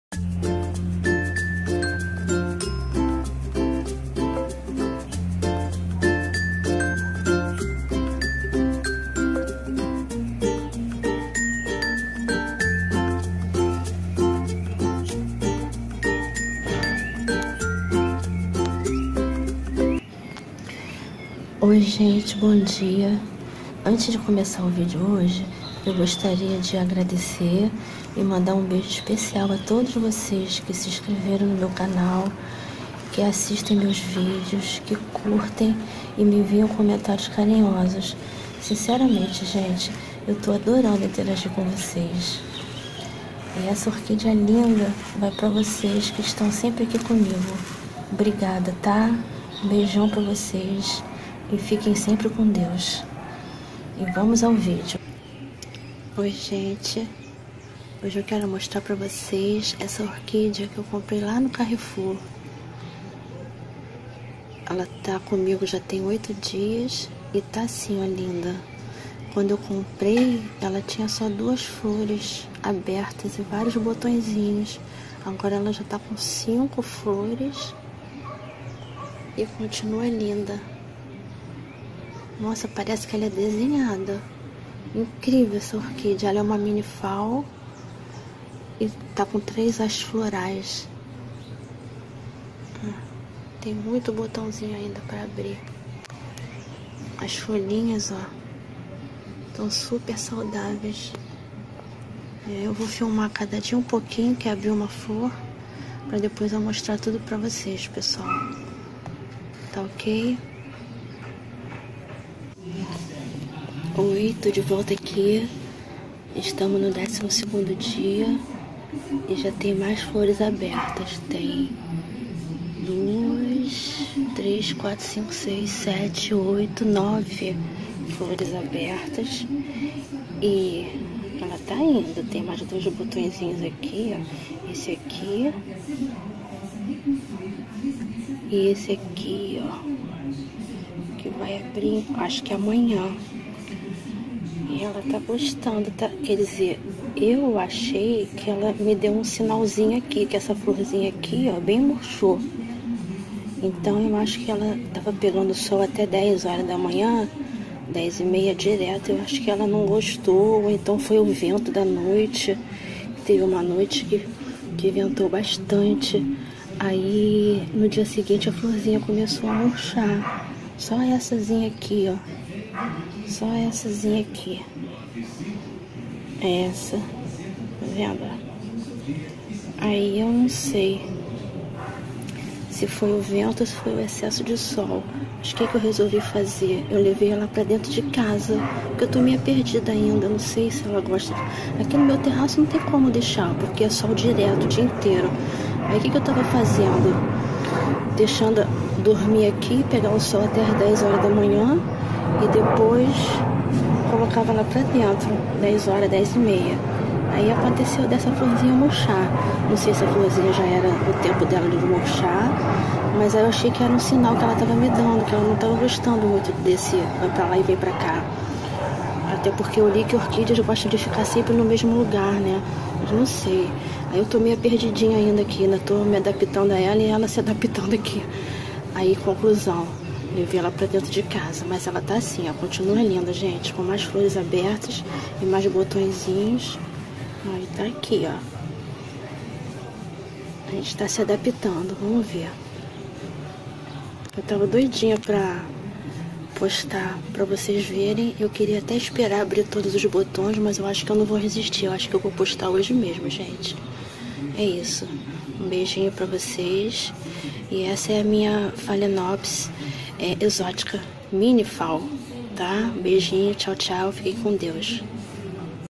Mini Orquídea Phalaenopsis Exótica
Oiii gente, essa é a minha mini phalaenopsis exótica... linda!
Estou tentando me adaptar a ela, acho que ela está gostando... mas ainda preciso aprender muito sobre as orquídeas, acho que com o tempo tudo vai dar certo.